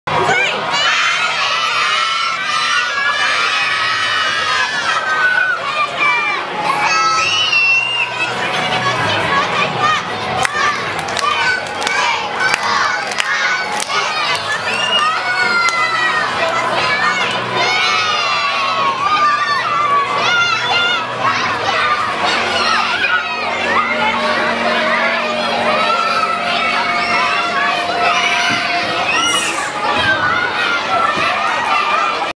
Lollie Pops Kids Party
The staff are really good at lollie pops, they put on good party for the kids and handle them all very well, feeding them red cordial and lollies until they are full, then we take them home